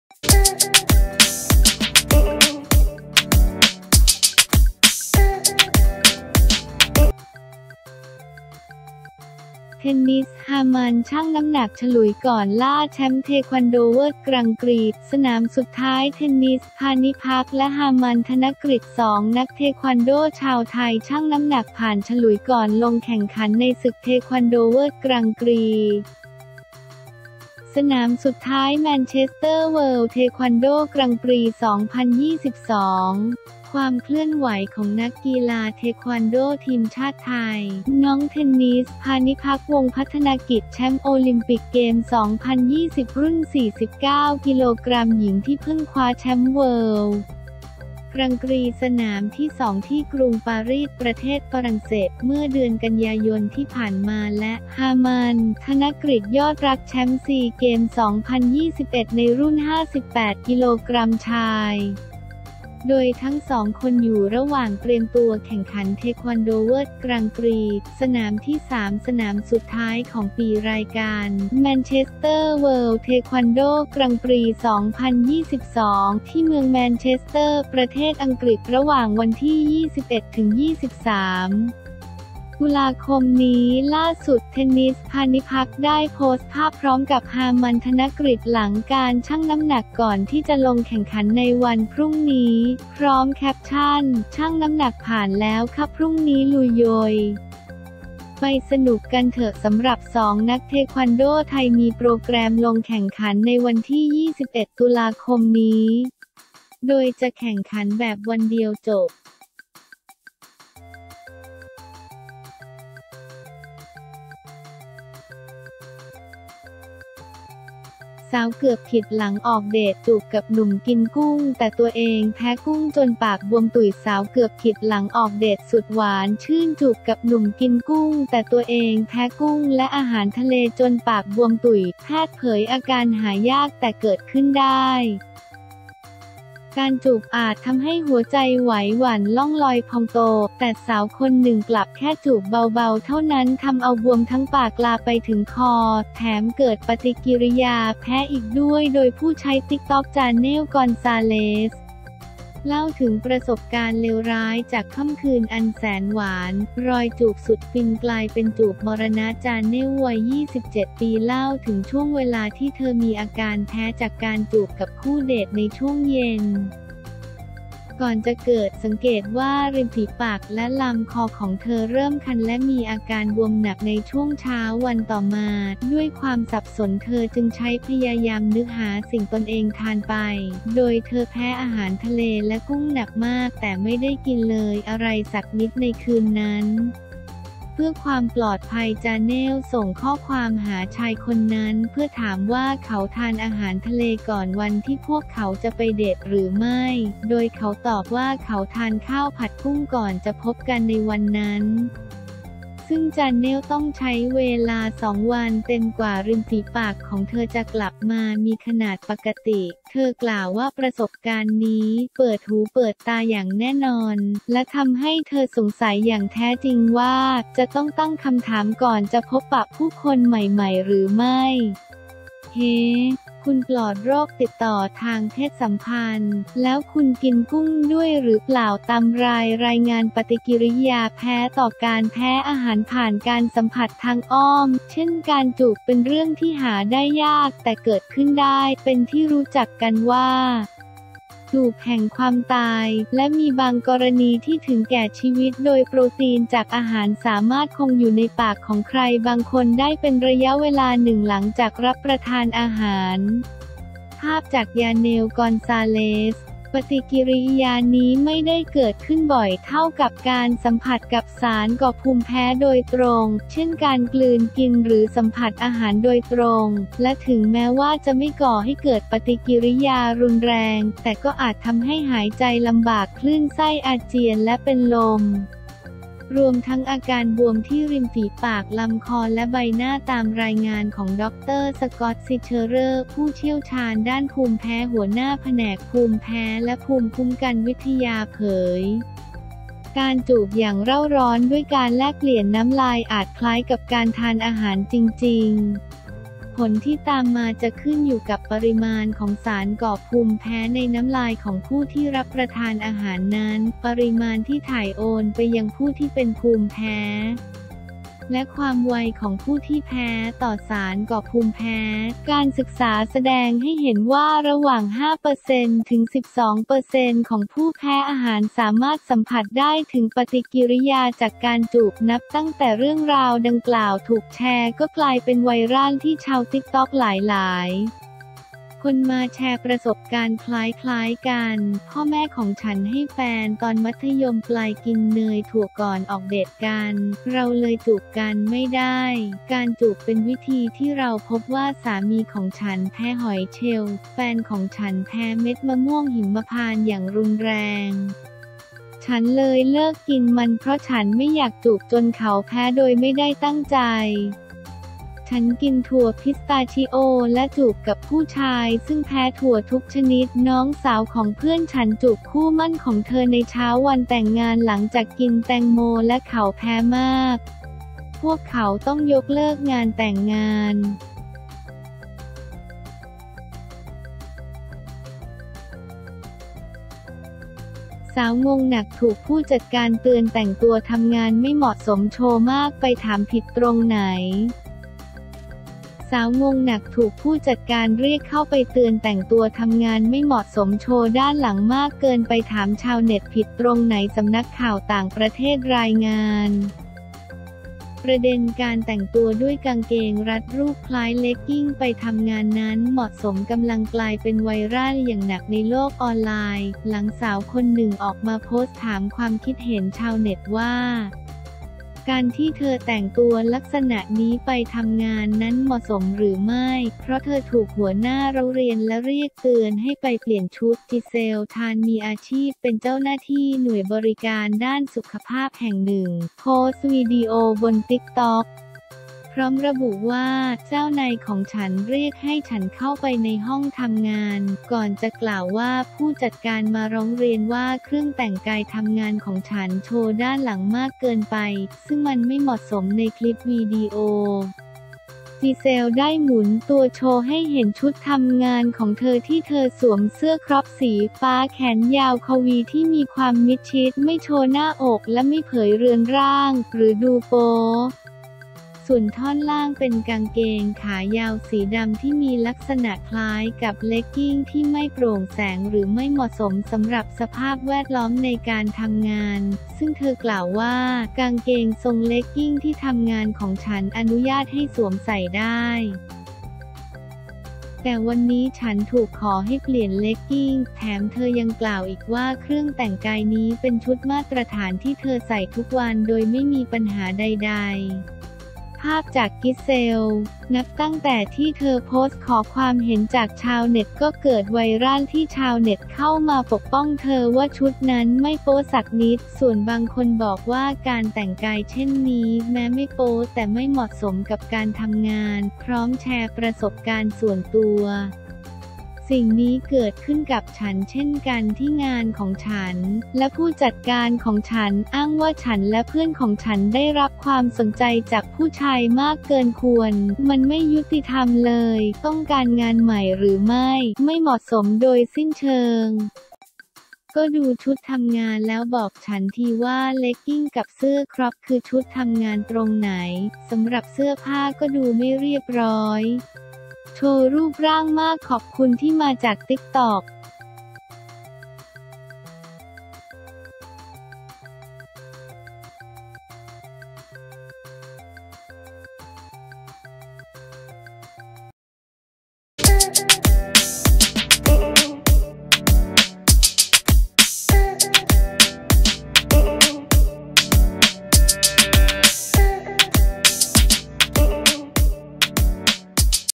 เทนนิส ฮามัน ชั่งน้ำหนักฉลุยก่อนล่าแชมป์เทควันโดเวิล์ดกรังด์กรีซ์ สนามสุดท้าย
เทนนิส พาณิภัค และ ฮามัน ธนกฤต 2 นักเทควันโดชาวไทย ชั่งน้ำหนักผ่านฉลุยก่อนลงแข่งขันในศึกเทควันโดเวิล์ดกรังด์กรีซ์ สนามสุดท้าย แมนเชสเตอร์ เวิลด์ เทควันโด กรังด์ปรีซ์ 2022
ความเคลื่อนไหวของนักกีฬาเทควันโดทีมชาติไทย “น้องเทนนิส” พาณิภัค วงศ์พัฒนกิจ แชมป์โอลิมปิกเกมส์ 2020 รุ่น 49 กก.หญิง ที่เพิ่งคว้าแชมป์เวิลด์ กรังกรีซ์ สนามที่ 2 ที่กรุงปารีส ประเทศฝรั่งเศส เมื่อเดือนกันยายนที่ผ่านมา และ “ฮามัน” ธนกฤต ยอดรักษ์ แชมป์ซีเกมส์ 2021 ในรุ่น 58 กก.ชาย โดยทั้งสองคนอยู่ระหว่างเตรียมตัวแข่งขันเทควันโดเวิล์ดกรังด์กรีซ์ สนามที่ 3 สนามสุดท้ายของปี รายการ “แมนเชสเตอร์ เวิลด์ เทควันโด กรังด์ปรีซ์ 2022” ที่เมืองแมนเชสเตอร์ ประเทศอังกฤษ ระหว่างวันที่ 21-23 ต.ค.นี้
ล่าสุด เทนนิส พาณิภัค ได้โพสต์ภาพพร้อมกับ ฮามัน ธนกฤต หลังการชั่งน้ำหนักก่อนที่จะลงแข่งขันในวันพรุ่งนี้ พร้อมแคปชั่น “ชั่งน้ำหนักผ่านแล้วค่ะ พรุ่งนี้ลุยยยยยย !! ไปสนุกกันเถอะ
สำหรับสองนักเทควันโดไทย มีโปรแกรมลงแข่งขันในวันที่ 21 ตุลาคมนี้ โดยจะแข่งขันแบบวันเดียวจบ